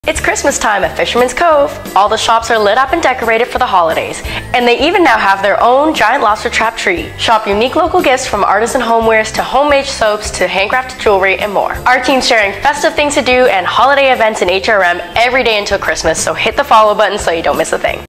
Christmas at the Cove
As if the colourful shops of Fisherman’s Cove couldn’t get any better..they’re now all lit up for Christmas 🎄

Shop unique gifts, grab a bit to eat and see their new giant lobster trap tree!

🗓️Thursdays-Saturdays 12-6pm & Sundays 12-5pm
📍Fisherman’s Cove, Eastern Passage

Celebrate the season with our Team’s 24DaysofHRMFestivities ✨

Find more info on HRM festivities on our blog at ThinkHalifax.com